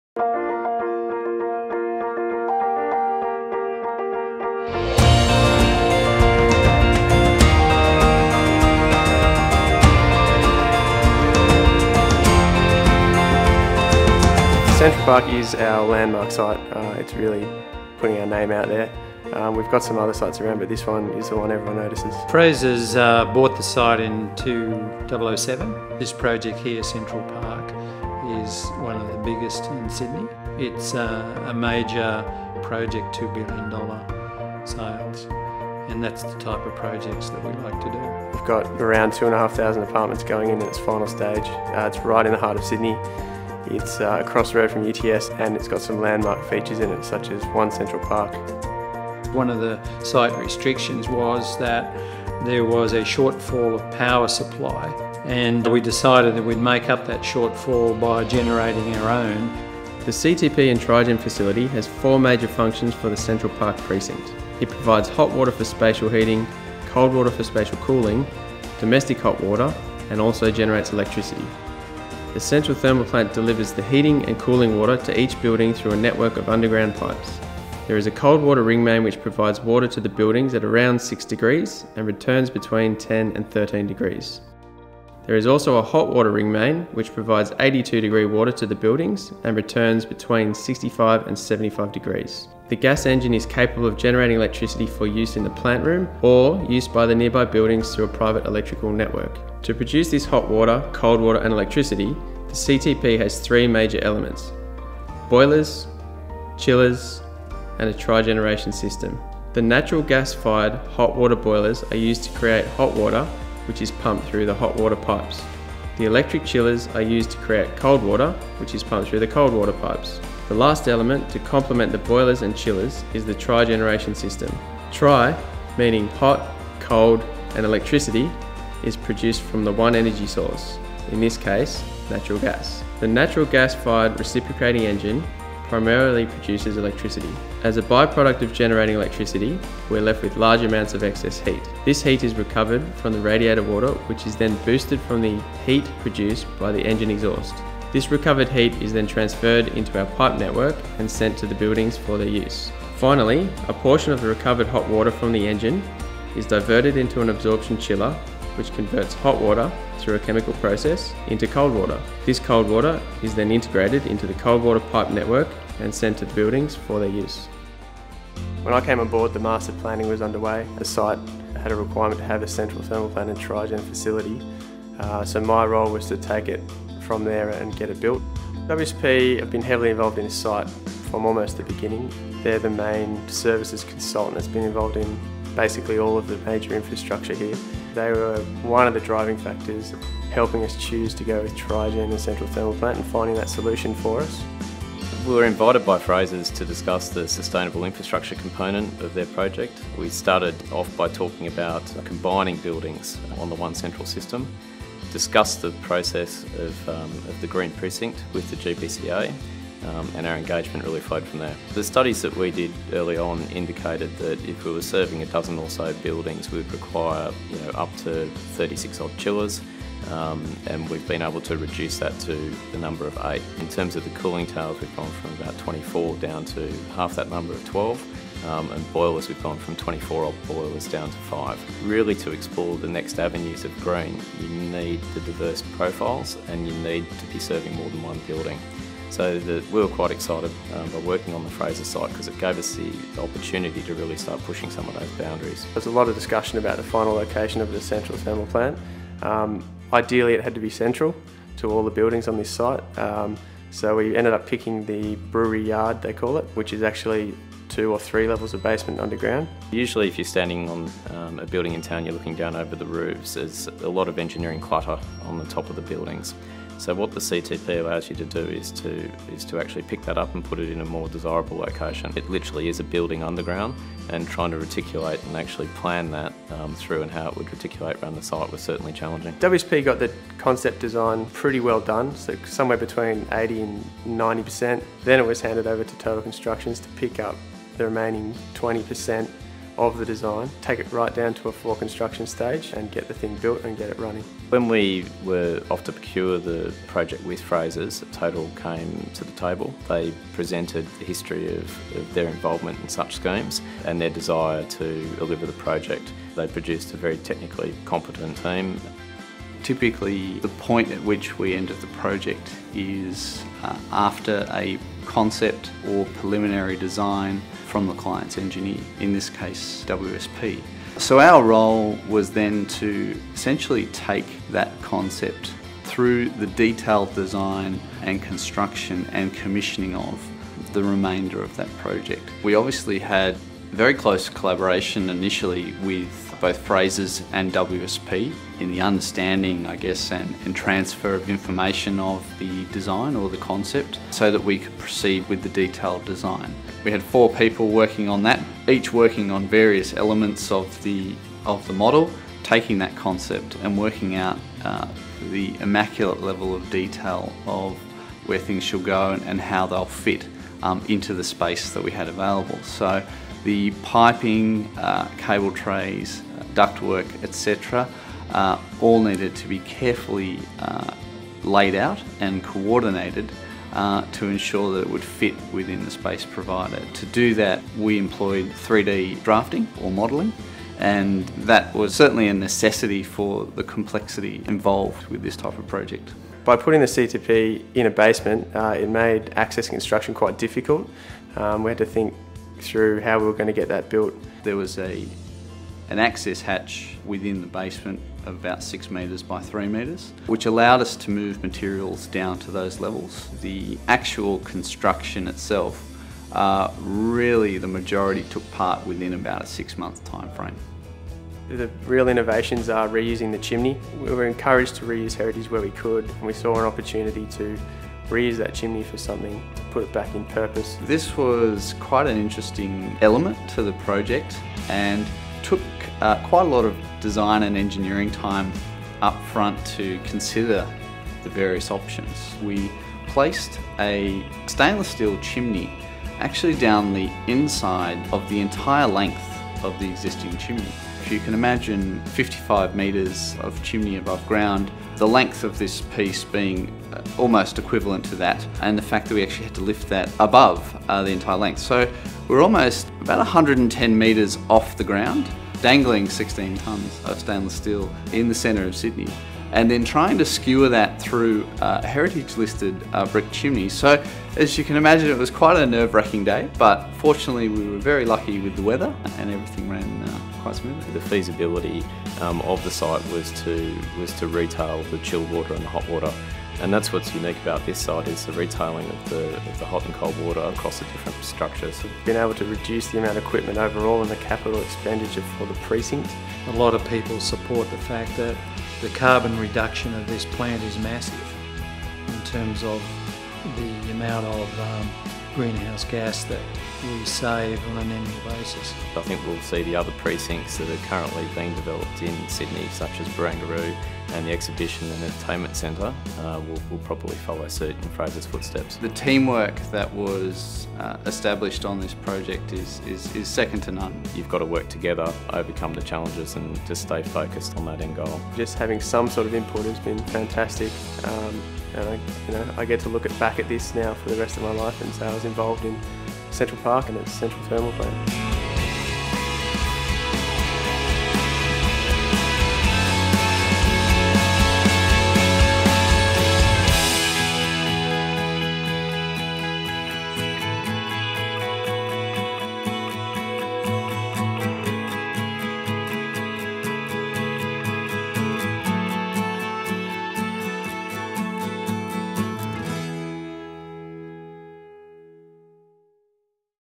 Central Park CTP Video - long version
The Central Park CTP is a path-leading project for urban utilities,  changing the paradigm for our management of urban energy and  water networks. Testing the governance structures of energy market regulation, it has broken new ground in the reticulation and commercialisation of thermal energy networks in Australia.

It has delivered on the triple-bottom line, engineering that serves  people, profit and planet; an exemplar project for urban utility infrastructure.

WSP has provided sustainability and engineering support for the CTP from its earliest concept stages through to the completion of construction and energy supply commercial negotiations.